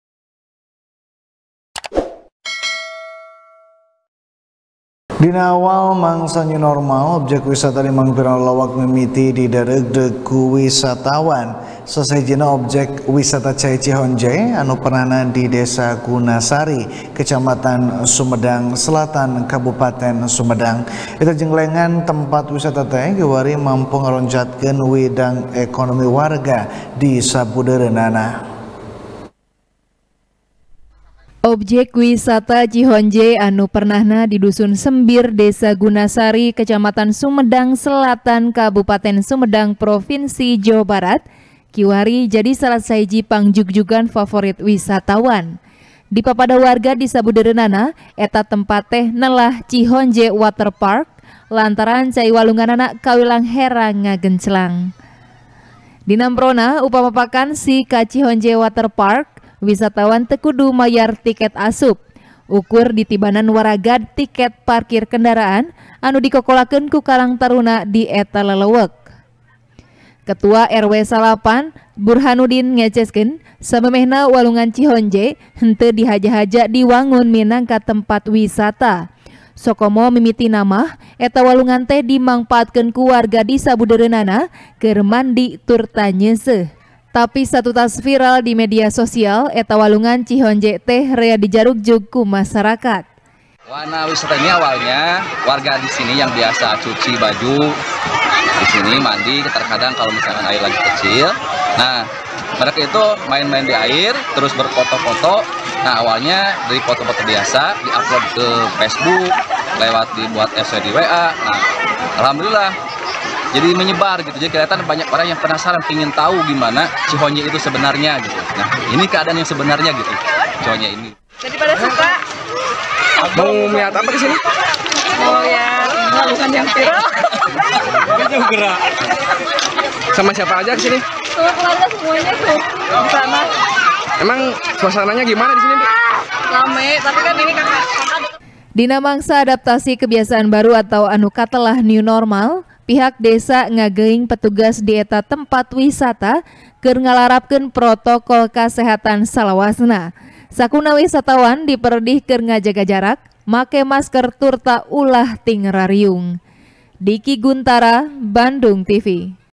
Viral, Tempat Wisata Cihonje Anu Unik | TP | BANDUNGTV
TP | BANDUNGTV | DERI FIRMAN | Dina Awal Mangsa New Normal, Objek Wisata Di Mangpirang Lelewek Mimiti Didareugdeug Ku Wisatawan, Salah Sahijina Objek Wisata Cai Cihonje, Anu Pernahna Di Desa Gunasari, Kecamatan Sumedang Selatan, Kabupaten Sumedang. Eta Jenglengan Tempat Wisata Teh, Kiwari, Mampuh Ngaronjatkeun Widang Ekonomi Warga Di Sabudeureunana.